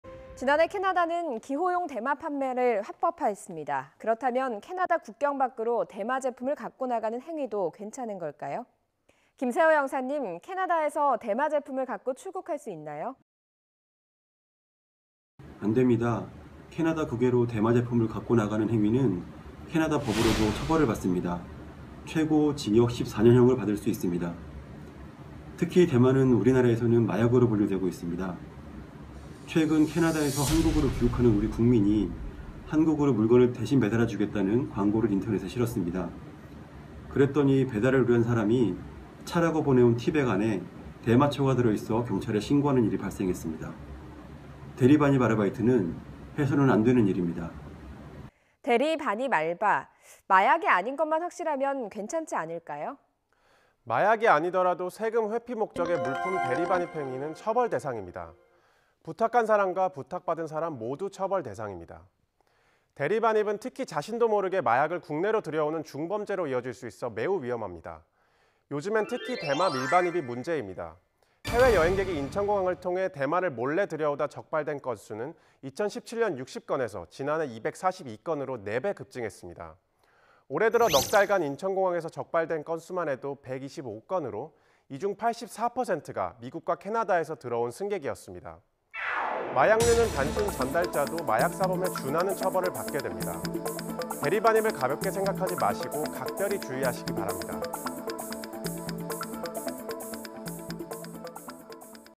캐나다, 남의 물건 운반하다간 징역형 / YTN KOREAN
[앵커]
지난해 캐나다는 기호용 대마 판매를 합법화했습니다.

그렇다면 캐나다 국경 밖으로 대마 제품을 갖고 나가는 행위도 괜찮은 걸까요?

김세호 영사님, 캐나다에서 대마 제품을 갖고 출국할 수 있나요?

[김세호 / 주캐나다 대한민국 대사관 영사]
안 됩니다.

캐나다 국외로 대마 제품을 갖고 나가는 행위는 캐나다법으로도 처벌을 받습니다.

최고 징역 14년형을 받을 수 있습니다.

특히 대마는 우리나라에서는 마약으로 분류되고 있습니다.

최근 캐나다에서 한국으로 귀국하는 우리 국민께서 한국으로 물건을 배달해주겠다는 광고를 인터넷에 실었습니다.

그랬더니 배달을 의뢰한 사람이 차(茶)라고 보낸 티백 안에 대마초가 들어있어 경찰에 신고하는 일이 발생했습니다.

대리 반입 아르바이트는 해서는 안 되는 일입니다.

[앵커]
대리 반입 알바, 마약이 아닌 것만 확실하면 괜찮지 않을까요?

[사무관]
마약이 아니더라도 세금 회피 목적의 물품 대리 반입 행위는 처벌 대상입니다.

부탁한 사람과 부탁받은 사람 모두 처벌 대상입니다.

대리 반입은 특히 자신도 모르게 마약을 국내로 들여오는 중범죄로 이어질 수 있어 매우 위험합니다.

요즘엔 특히 대마 밀반입이 문제입니다.

해외 여행객이 인천공항을 통해 대마를 몰래 들여오다 적발된 건수는 2017년 60건에서 지난해 242건으로 4배 급증했습니다.

올해 들어 넉 달간 인천공항에서 적발된 건수만 해도 125건으로 이 중 84%가 미국과 캐나다에서 들어온 승객이었습니다.

마약류는 단순 전달자도 마약 사범에 준하는 처벌을 받게 됩니다.

대리 반입을 가볍게 생각하지 마시고 각별히 주의하시기 바랍니다.